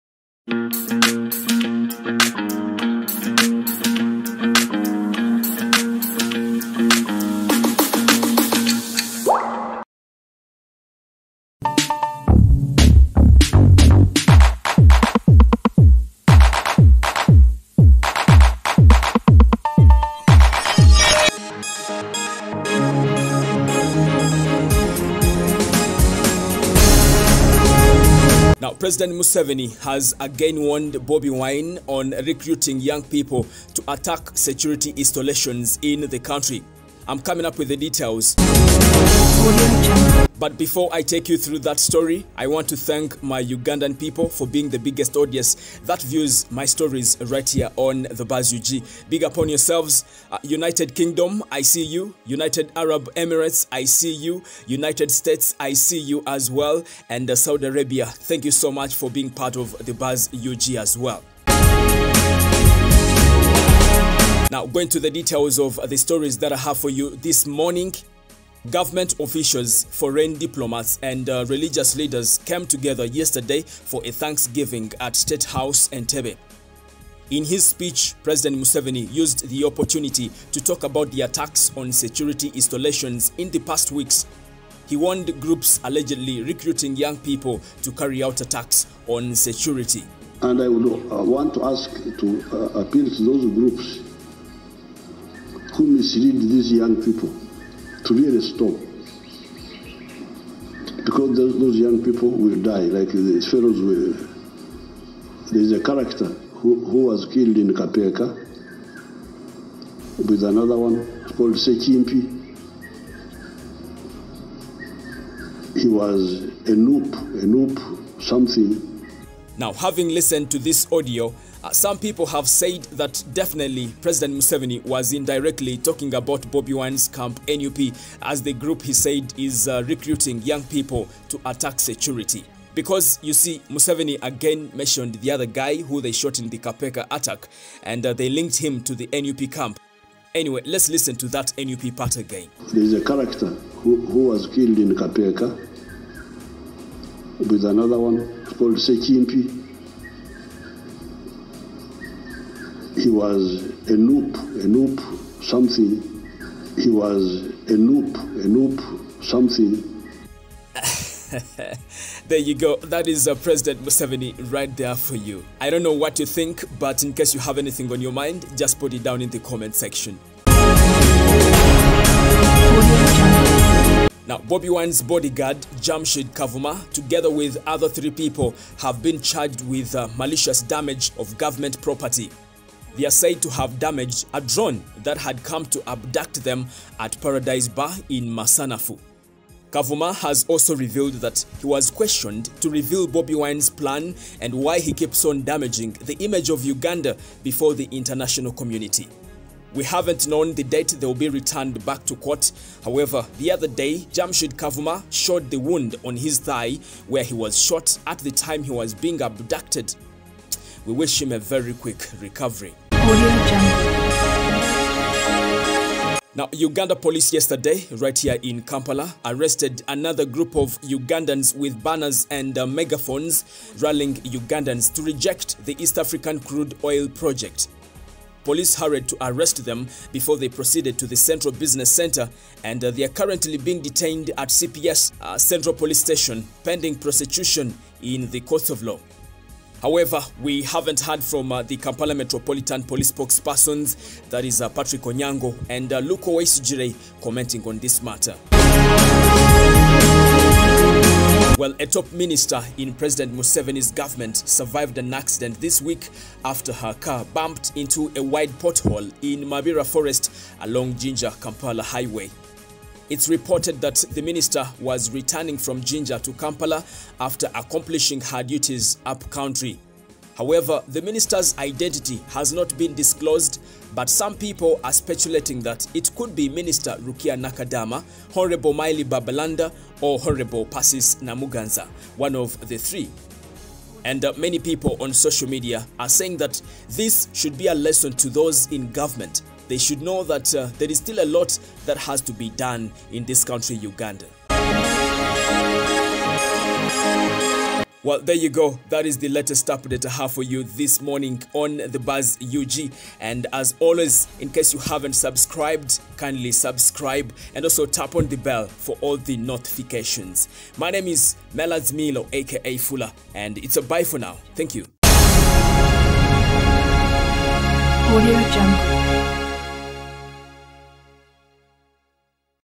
Bobi Wine stop recruiting young people to attack security, I will kill them! M7 again warns Bobi W..
Bobi Wine stop recruiting young people to attack security, I will kill them! Museveni again warns Bobi Wine